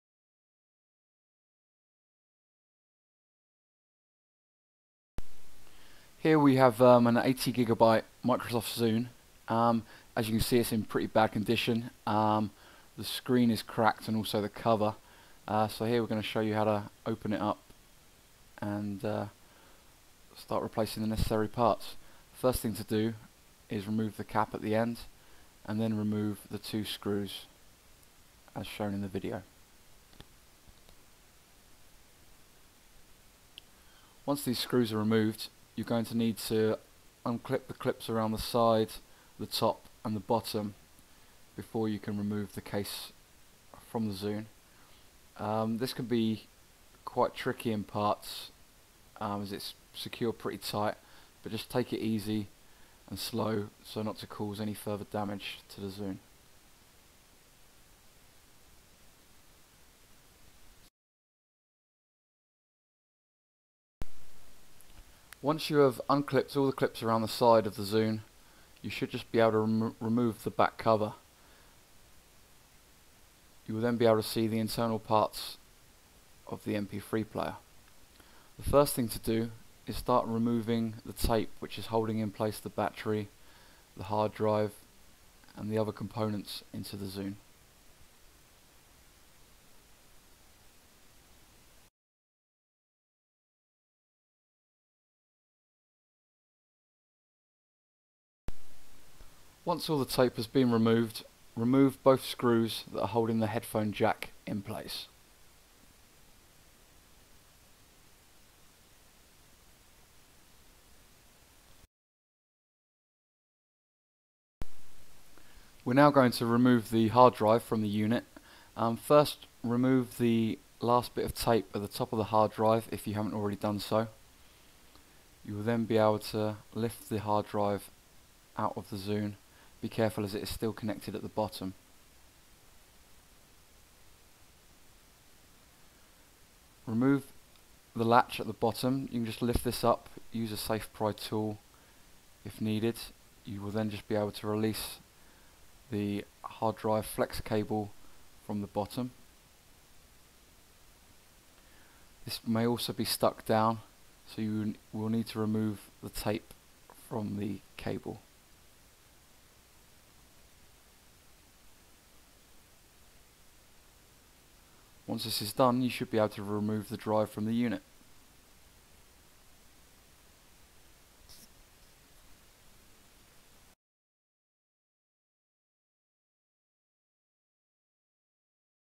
How to open and replace Zune 80GB Hard Drive / Disk
- This video shows how to open the 80GB Microsoft Zune and how to remove the hard drive. This may need to be done if the hard drive is faulty or damaged.

This part and others can be found on our website.